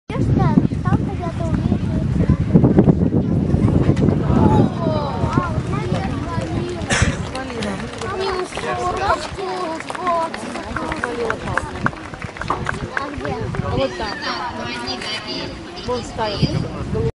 Процесс негативного подкрепления
На этих кадрах видно как жокей за плохо выполненный прыжок (а как я узнал - за этот прыжок довалось дофигищща баллов) дёргает со всей дури возжи, которые посредствам мунштука воздействуют на те места рта, где у лошади находится скопление нервных волокон, а потом ещё и шлёпает. Кстати что бы туда вставить этот самый мунштук лошади удаляют от туда зубы.